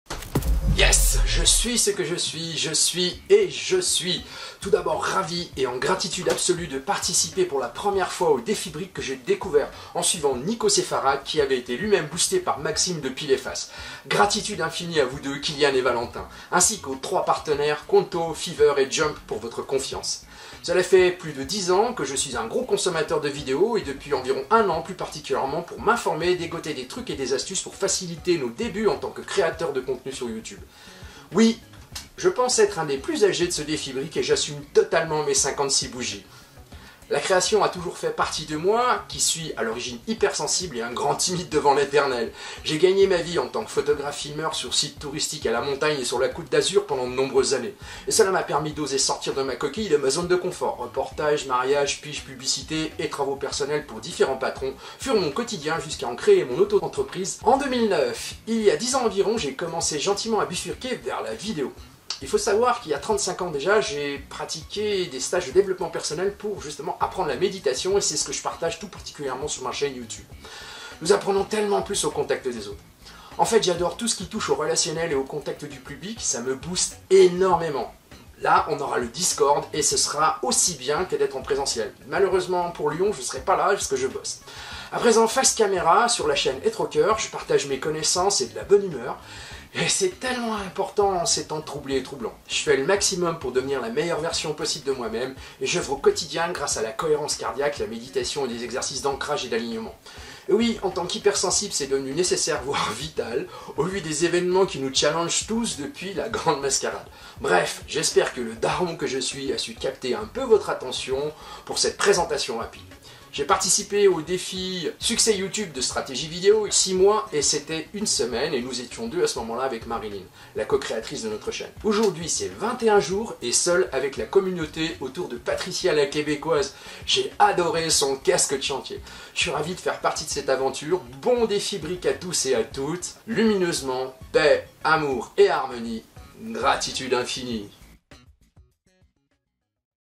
Présentation Êtreocoeur
Voici ma présentation pour le premier jour du Défi Briques 😀 🧱
Gratitude infinie à Valentin et Killian ainsi qu'aux trois partenaires
Qonto , Jump et Feverr !